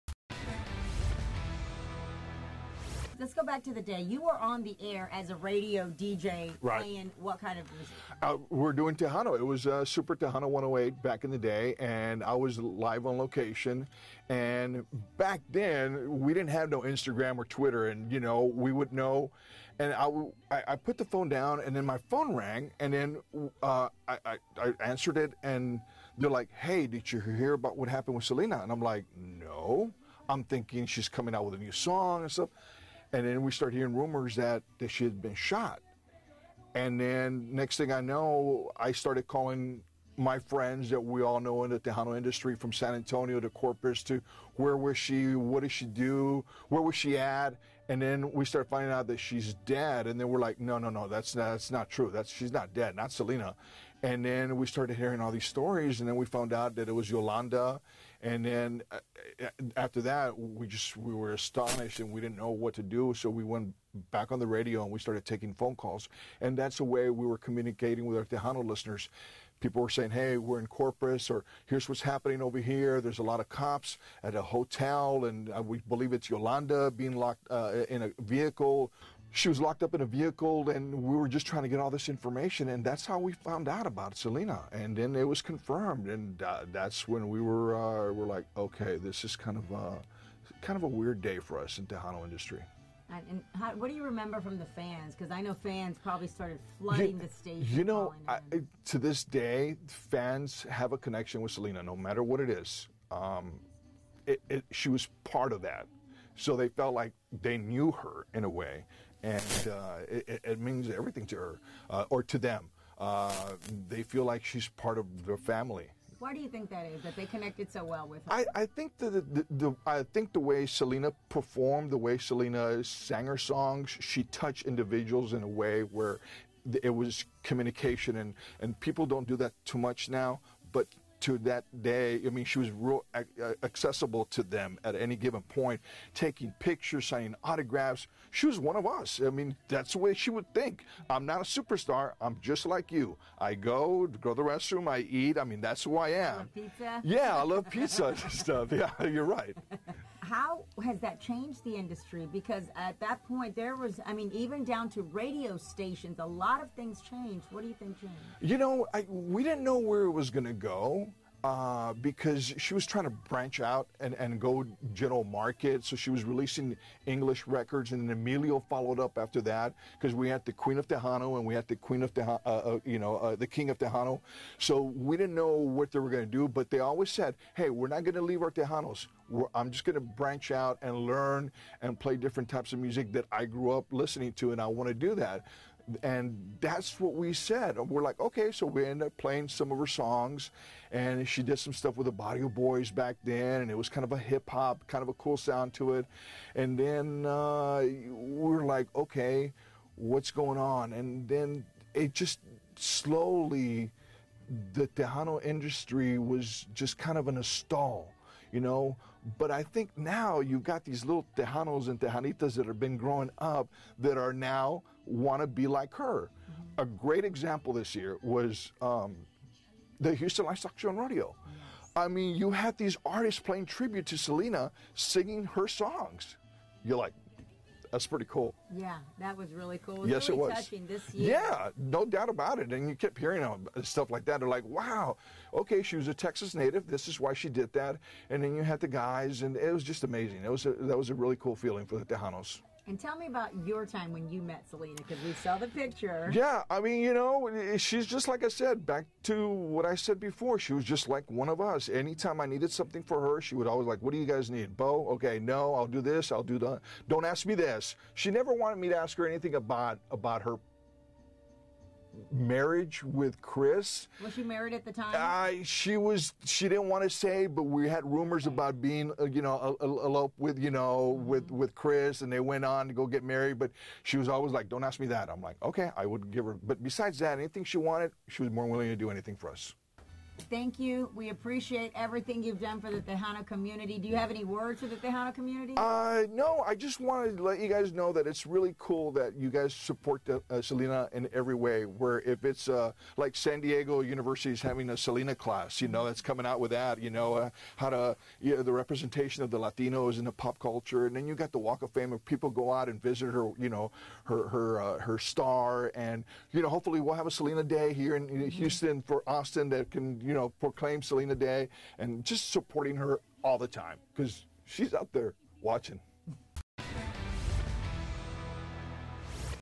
Bayou City Buzz bonus - veteran radio host Bo Corona describes the day he learned Selena was killed
FOX 26 The News Edge at 10 p.m. reporter CoCo Dominguez interviews Bo Corona.